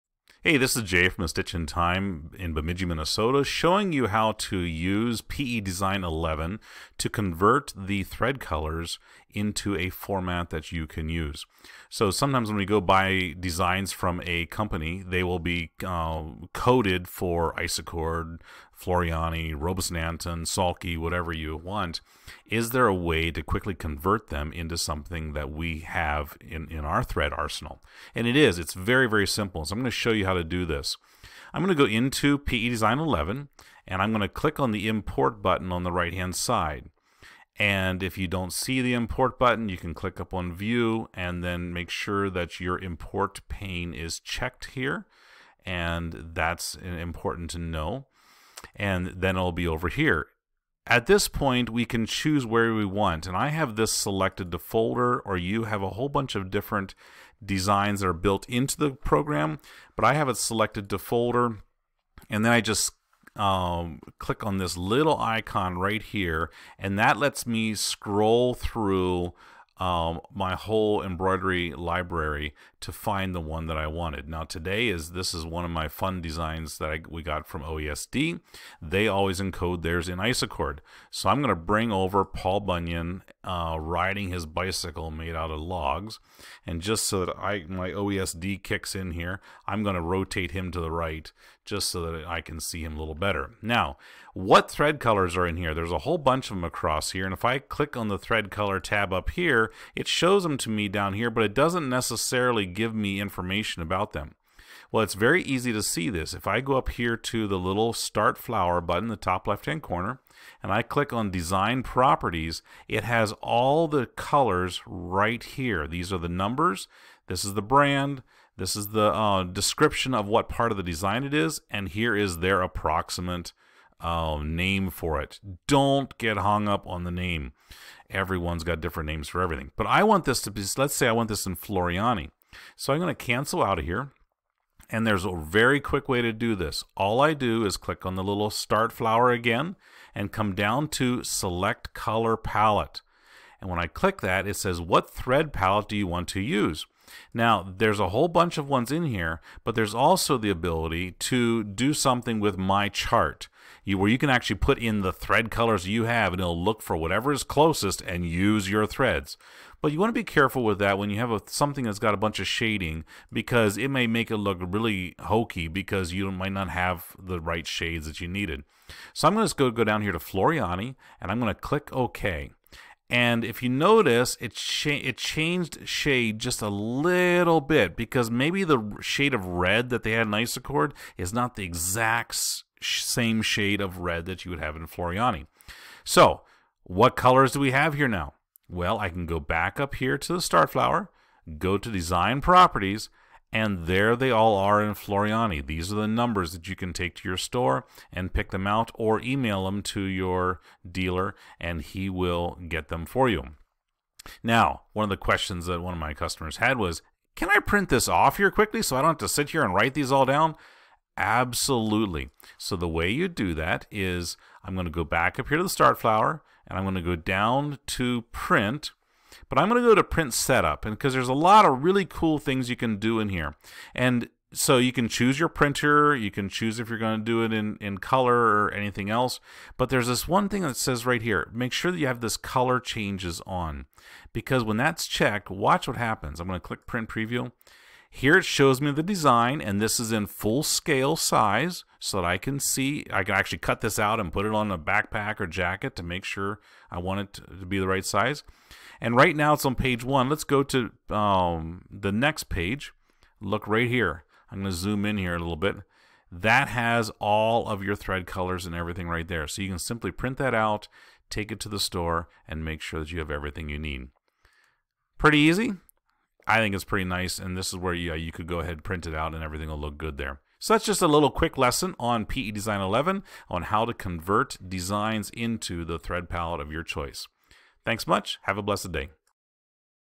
Auto Convert Thread colors in PE Design 11
In this Short video, I show you how to quickly convert all the thread colors in any design into the brand you prefer.   I also show you how to print out that thread list for easy reference.